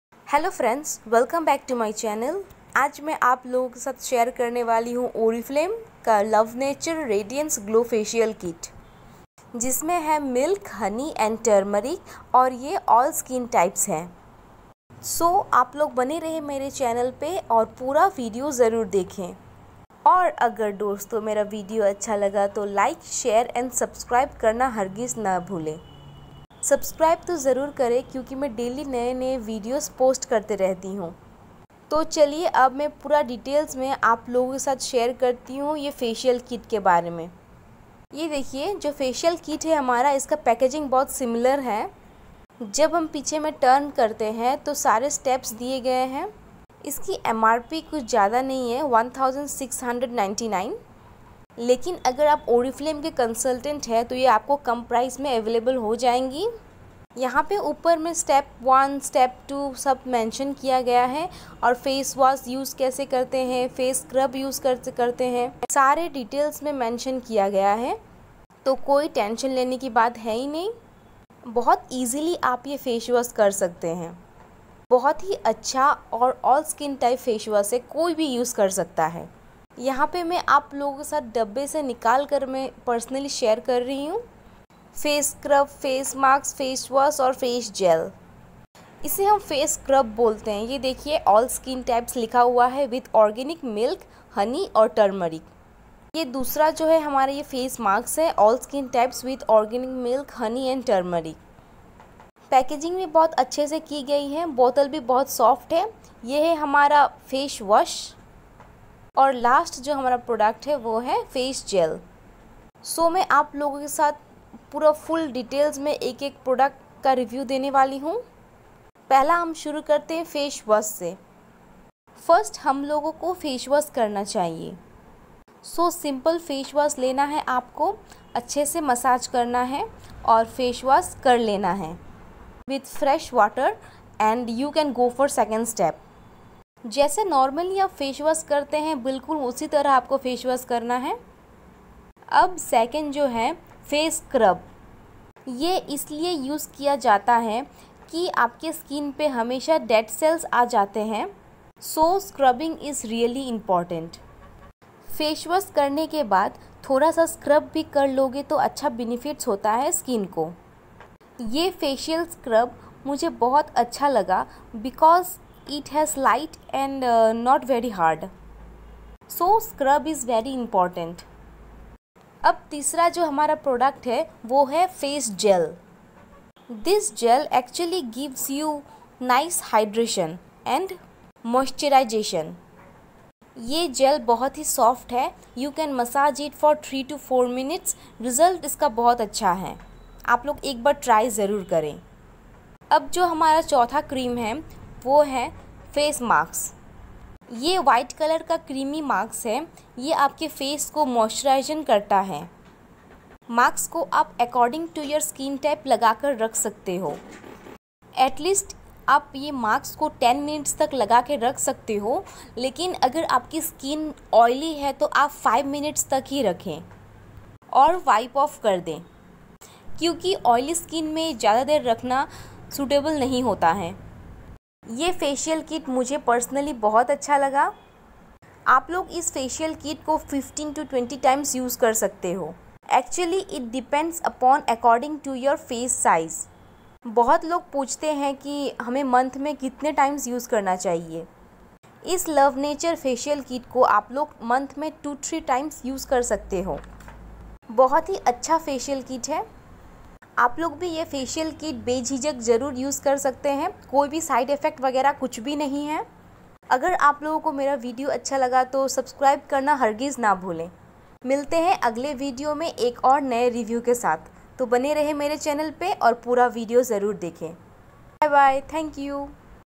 Oriflame Rediance Facial Kit Review|| कितने किट्स है| आपके लिए कौन सा अच्छा है| Iss it all skin type
Oriflame radiance facial kit review... facial kit kaise use Karen iske fayde aur everything available in this review